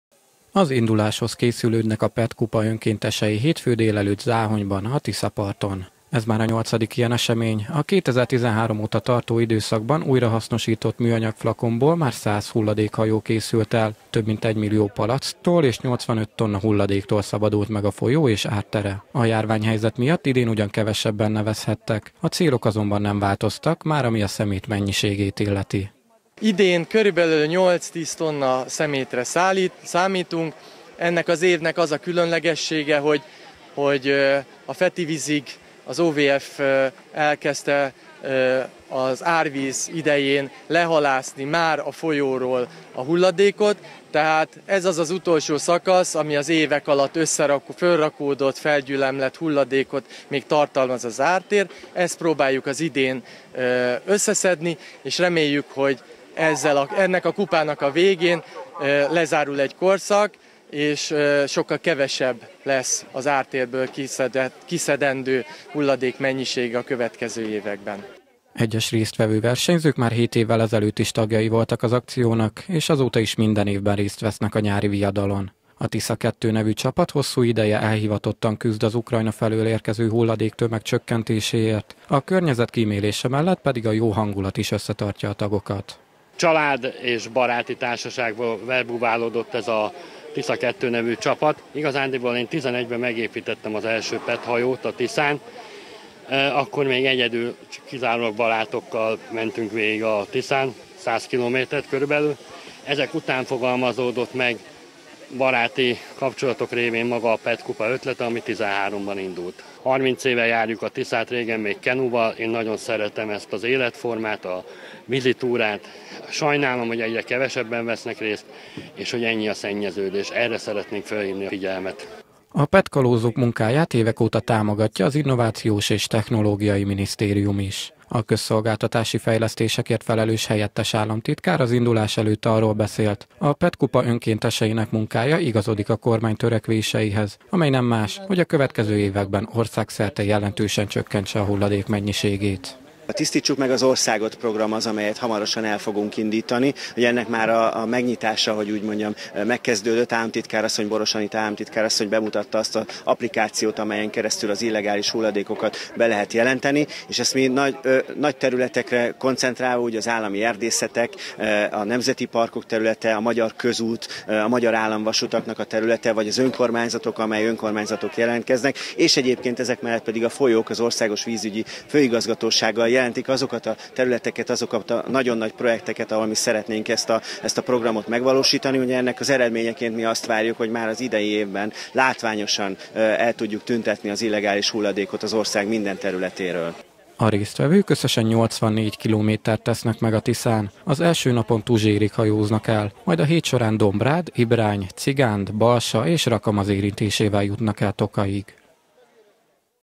Idén is elrajtolt a PET Kupa mezőnye - Kölcsey Televízió
A magyar kormány mindent megtesz azért, hogy minél kevesebb szennyező anyag legyen a Tiszában - mondta hétfőn Záhonyban Hizó Ferenc helyettes államtitkár.  Ma délelőtt tizenkét versenyhajóval elrajtolt az idei PET Kupa mezőnye. A mintegy 200 résztvevő augusztus első hetében főként műanyaghulladéktól igyekszik megtisztítani a Tisza Záhony és Tokaj közötti szakaszát. A verseny szombatig tart, a csapatok akár tíz tonnányi szemetet is kihalászhatnak a folyóból.